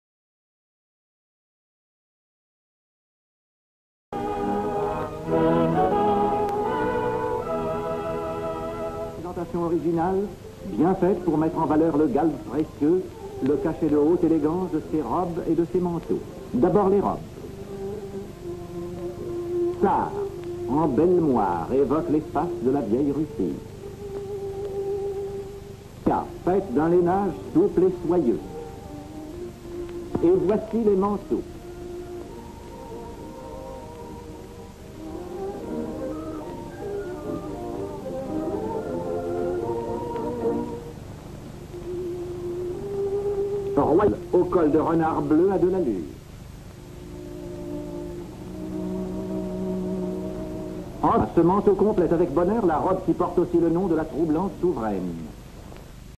Alcuni modelli invernali di vestiti e mantelli
Descrizione sequenze:due modelle sfilano con abiti da sera su un palco in una scenografia teatrale ; un'altra modella in abito da sera bianco entra ; segue la presentazione di altri modelli da sera ; una modella gira su se stessa mostrando l'arricciatura del vestito sulle spalle ; altra modella ; un gruppo di modelle entra in scena presentando vestiti e mantelline invernali ; una modella mostra il collo di pelliccia di un cappotto ; altra modella con un cappellino sorride ;